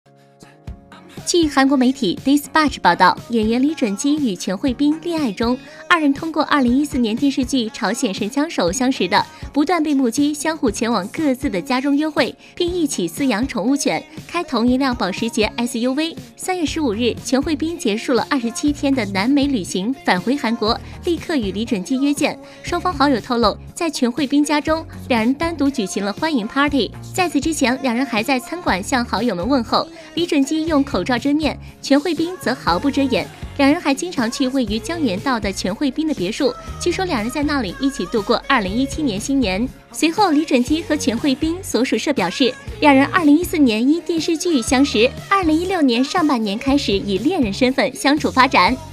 【3年前】韓媒曝李準基與全慧彬戀愛中 共赴別墅過新年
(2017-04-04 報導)

據韓媒報導，演員李準基與全慧彬戀愛中，二人通過2014年電視劇《朝鮮神槍手》相識的，不斷被目擊相互前往各自的家中約會，并一起飼養寵物犬，開同一輛保時捷SUV。3月15日，全慧彬結束了27天的南美旅行返回南韓，立刻與李準基約見。雙方好友透露“在全慧彬家中，兩人單獨舉行了歡迎party”，在此之前，兩人還在餐館向好友們問候，李準基用口罩遮面，全慧彬則毫不遮掩。兩人還經常去位于江原道的全慧彬的別墅，據說兩人在那裡一起度過2017年新年。